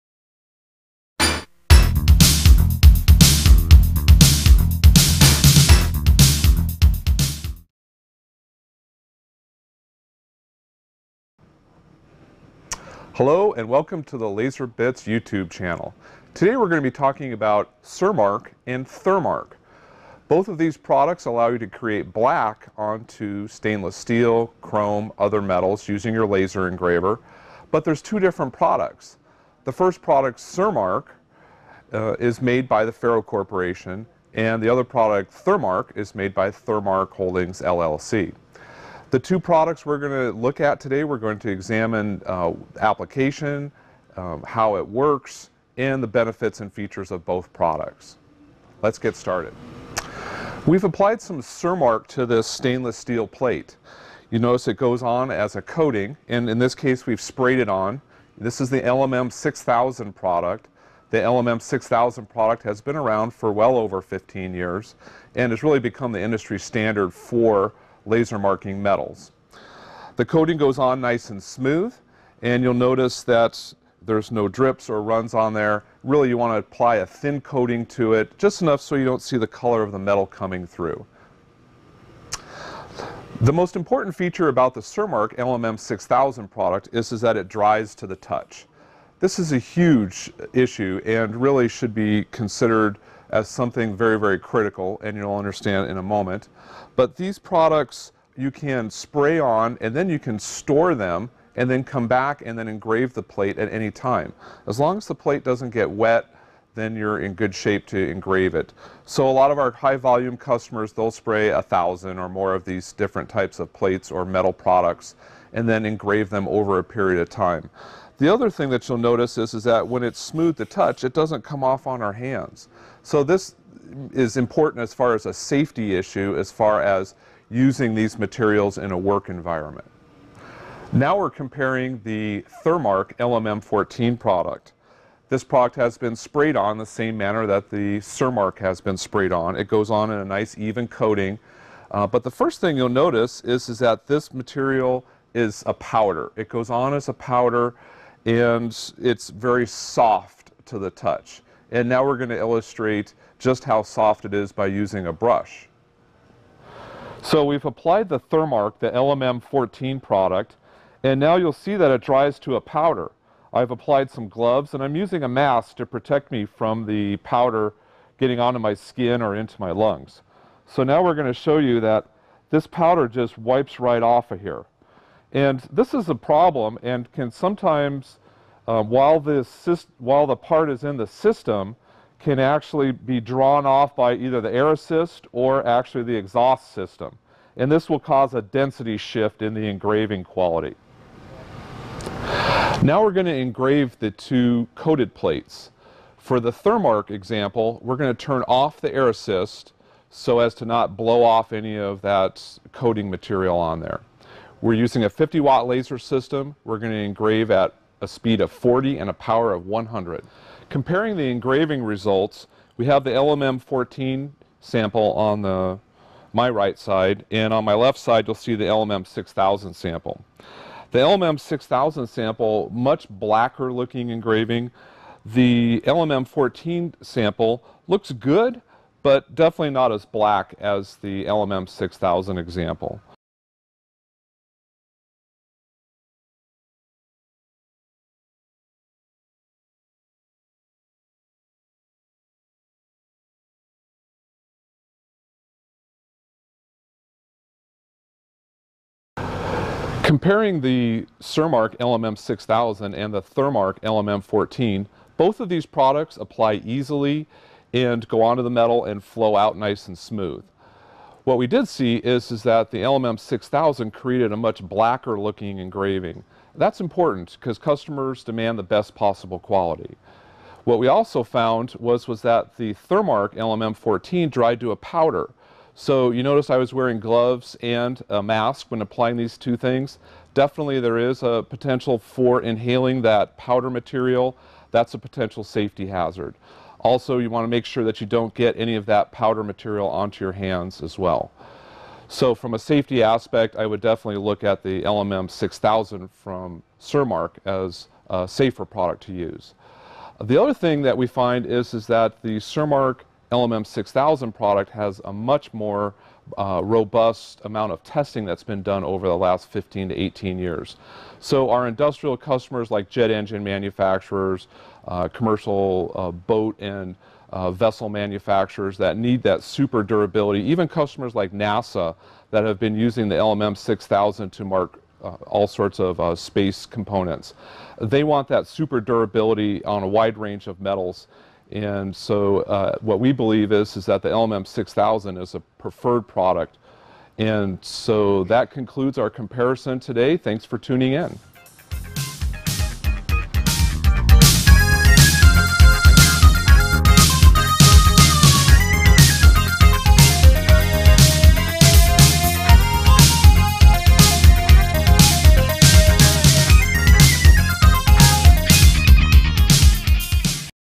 CerMark Thermark Comparison
- Which product is better CerMark or Thermark for metal marking? Is there a difference? This video outlines how different CerMark and Thermark are and which can yield a better result when metal marking your stainless steel or other uncoated metals. It even works for photo engraving!